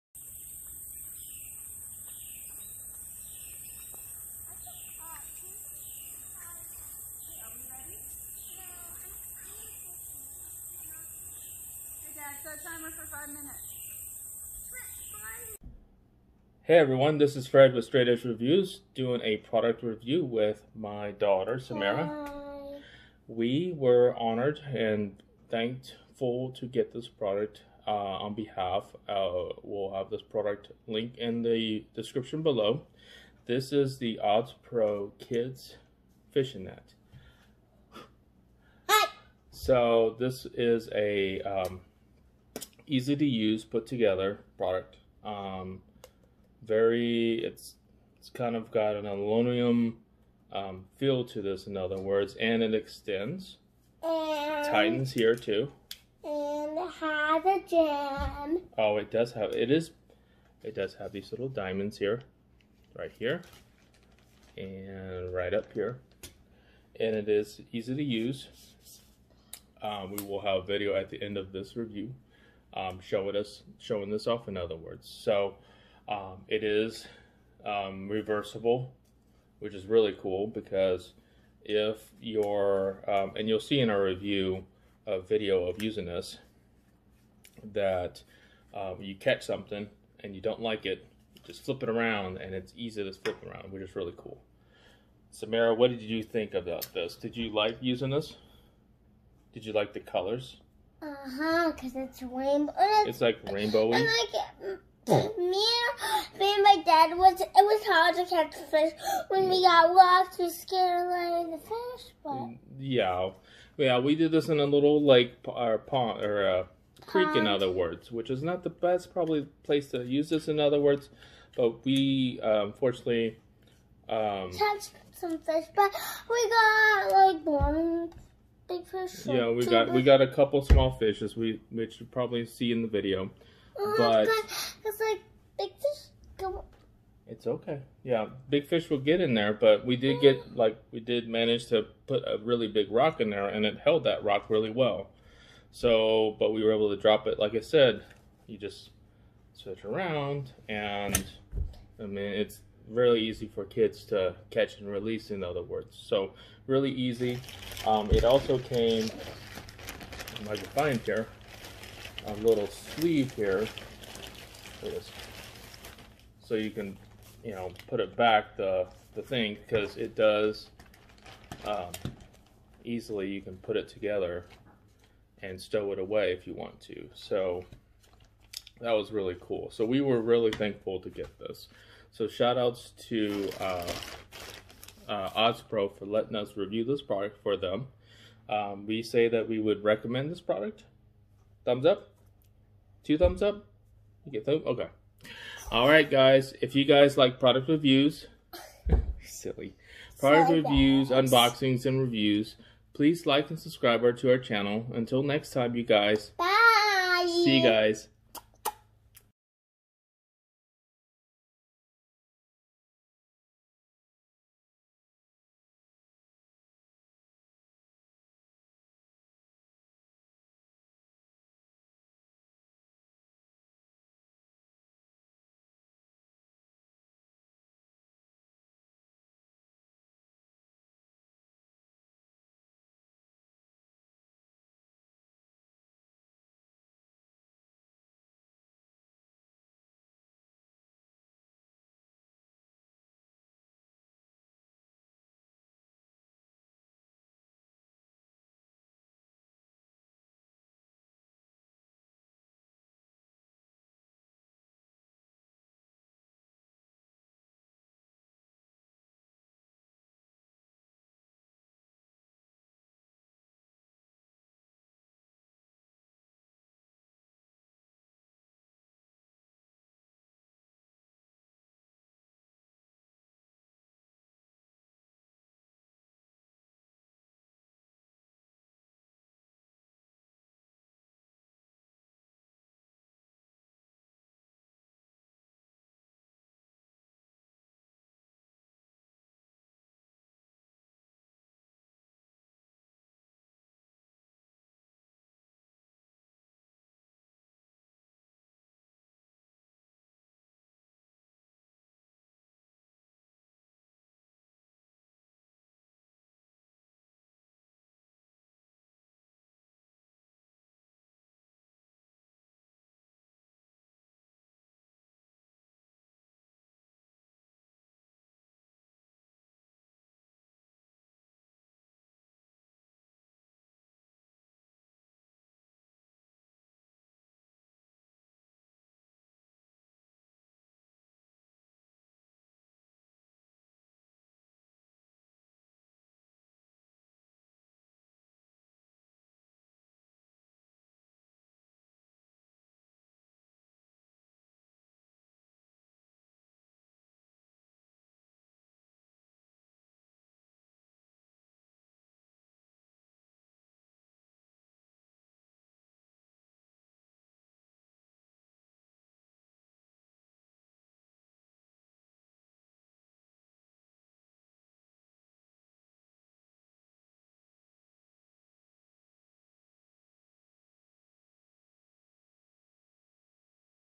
Product Review: OddsPro Kids Fishing Net
We review the OddsPro Kids Fishing Net, found on Amazon